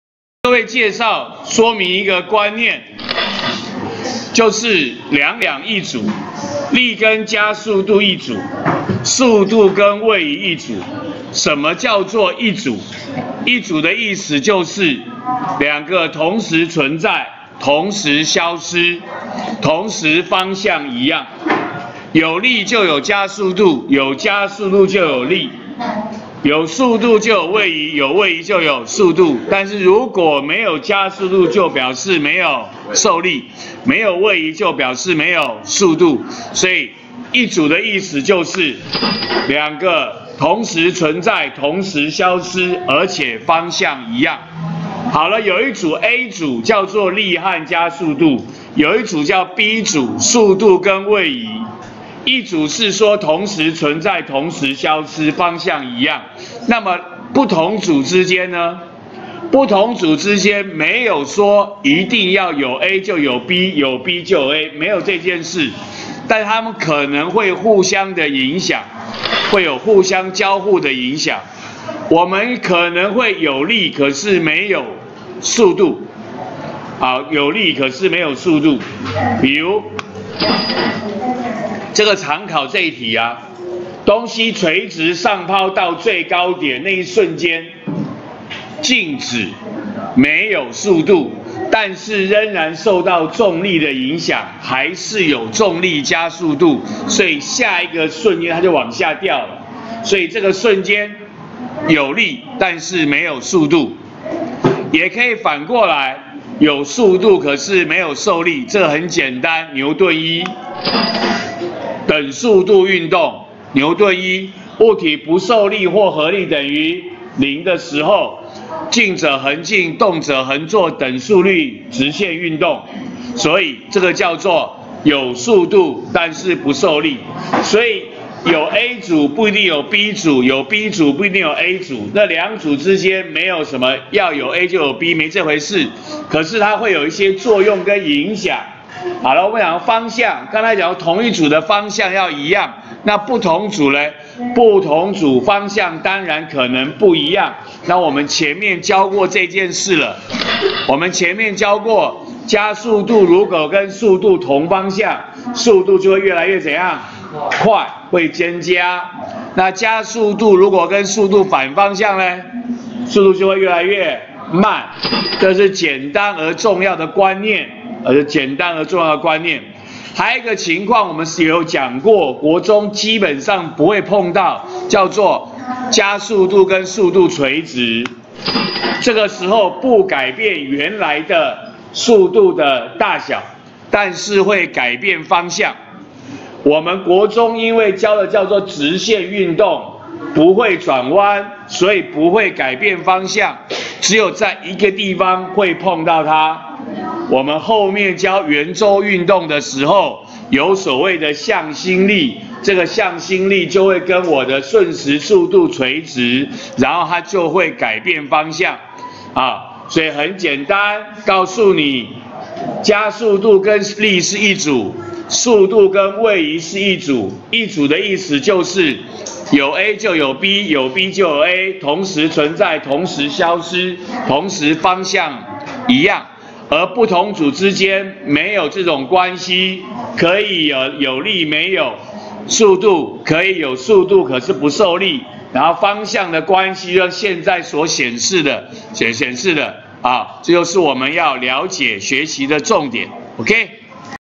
106學年國三理化＿牛頓運動定律20：兩組物理量與交互影響
指出力和加速度一組，速度和位移一組。說明何謂一組，同組間有何關係，兩組間有何交互作用。